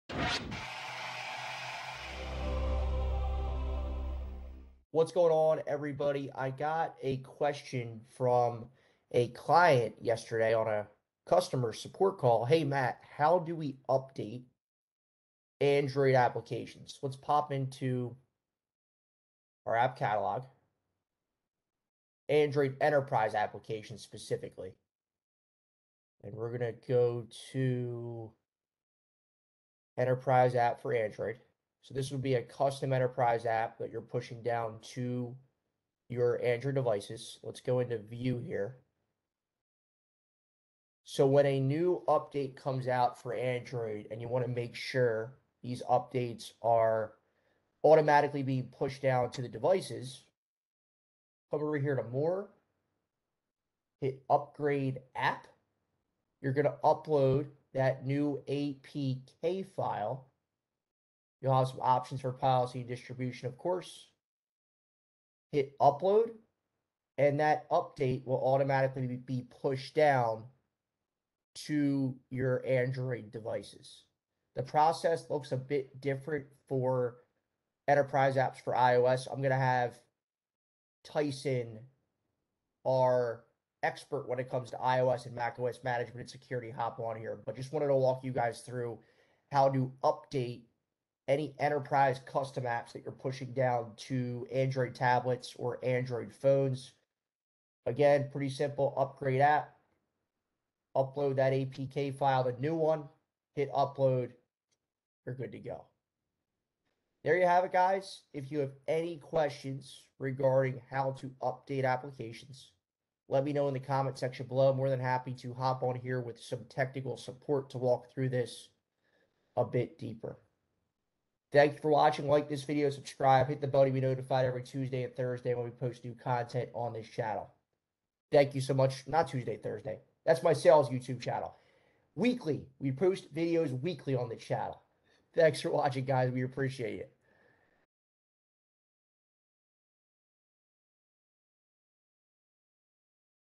How to Update Android Enterprise Applications with MaaS360
In this video, I walk through how to update your enterprise android apps.